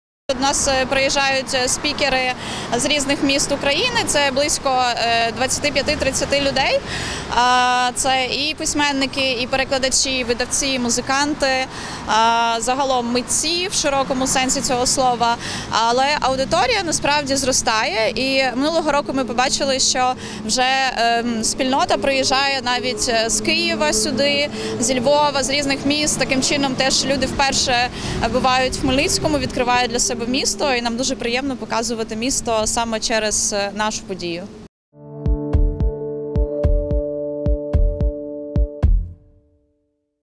Пресконференція з організаторами 3 фесту TRANSLATORIUM відбудеться сьогодні о 12 годині.
Пресконференція з учасниками та організаторами третього літературно-перекладацького фестивалю TRANSLATORIUM, під час якої детальніше розкажуть про процес створення цьогорічного фестивалю та розкриють деякі таємниці, відбудеться сьогодні о 12 годині. Про це розповіла директорка фестивалю Тетяна Родіонова. За її словами, цьогоріч фестиваль триватиме з 4 до 6 жовтня.